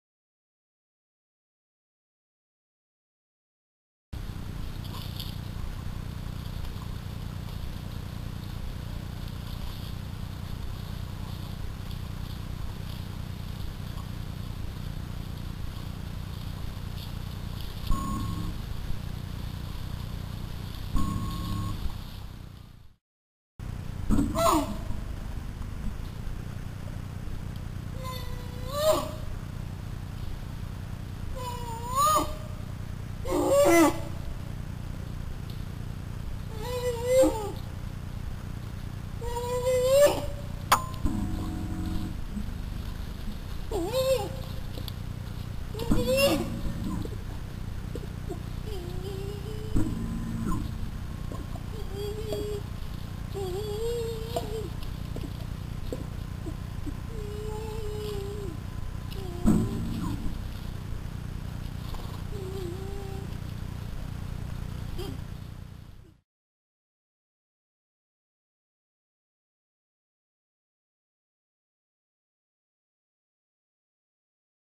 Lost Bear cub crying for mom
This is one of 3 Bear cubs that visited one of the deer feeders here with their mom. This cub was on the right side of the deer feeder when mom and the other 2 cubs left. After a while, this cub realized he was alone and began walking around looking for them. After about 10 minutes of looking around he began to get a little frantic and began crying loudly for them. Fortunately, there was a happy ending. We watched this live on Hidden Pond cam at USeeWildlife.com on August 20th, 2009. Join us at USeeWildlife.com to watch this and more LIVE.